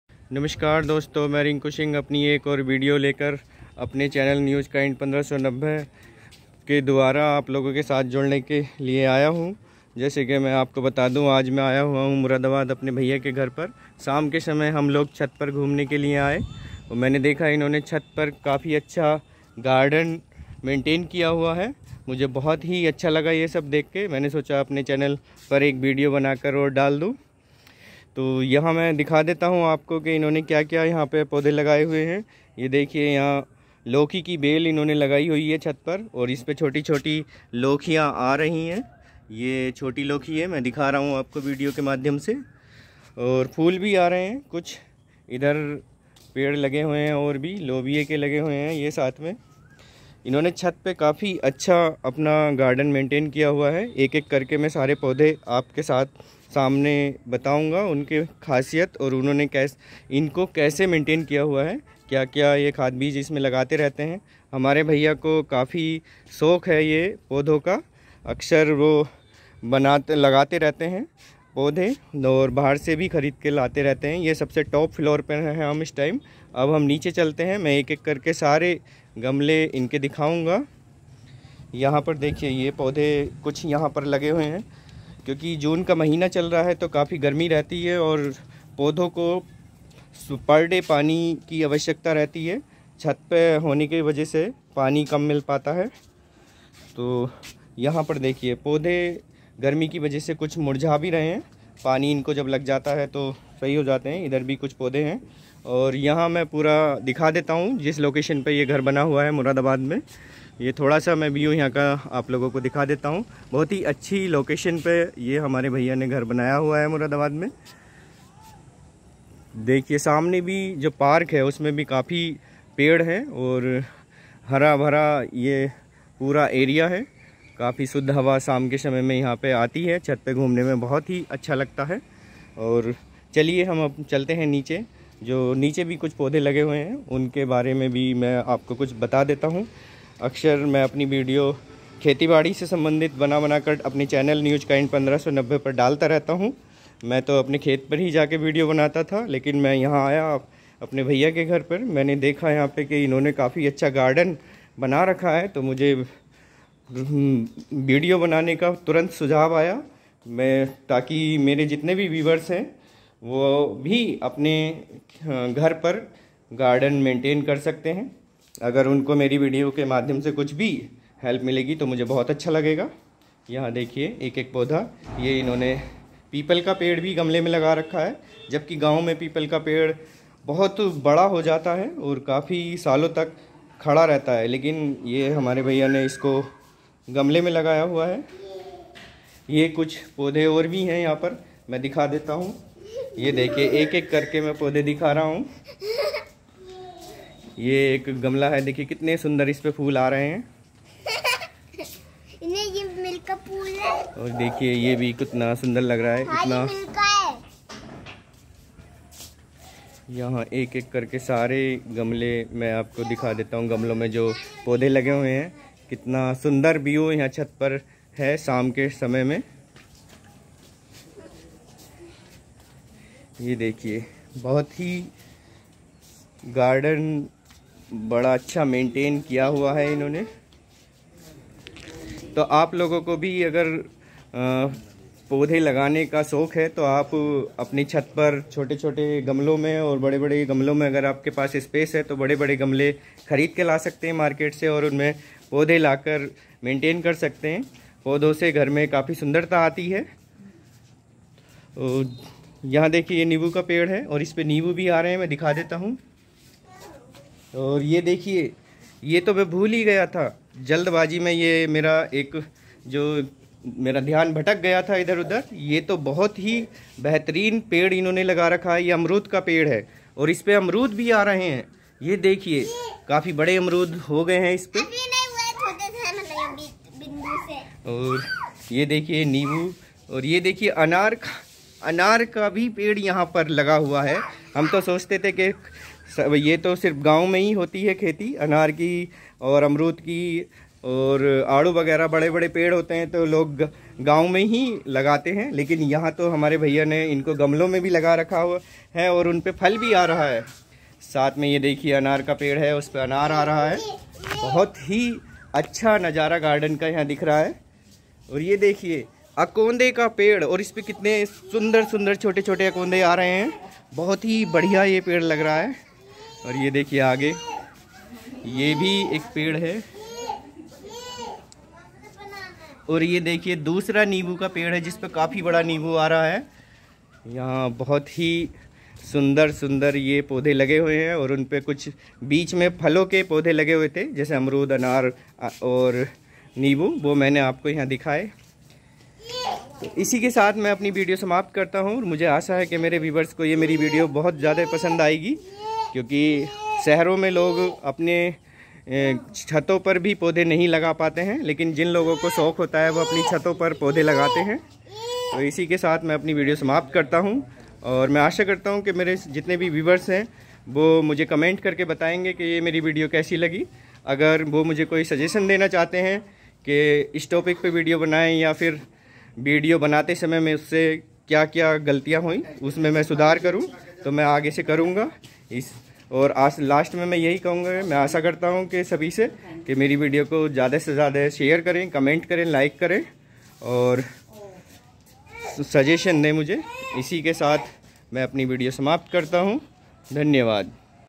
Gardon on Roof Top#viral video#youtube#
Ghar ki chhat par Gardon mei lage amrud,loki,aanar or neebu ke ..